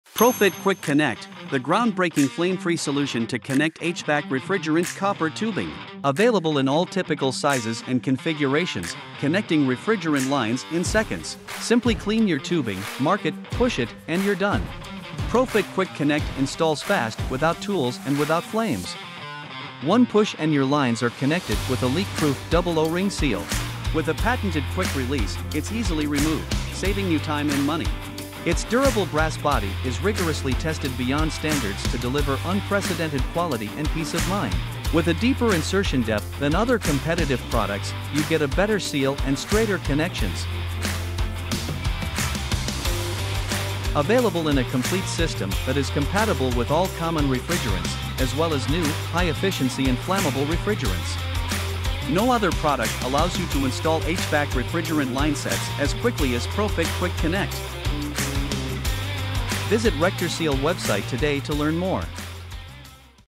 PRO-Fit Quick Connect Flame-Free Push-to-Connect HVAC Fittings Overview
PRO-Fit Quick Connect is the revolutionary push-to-connect refrigerant fitting solution that quickly and securely connects copper and coated aluminum linesets for mini-split and unitary HVAC units. With no flaring or brazing required, our unique leak-proof double O-rings sealing design creates a permanent high pressure gas seal that eliminates leaks caused by galling, vibration, pulsation and extreme temperature changes. Upgrade to PRO-Fit Quick Connect today - the perfect solution for your refrigerant line needs!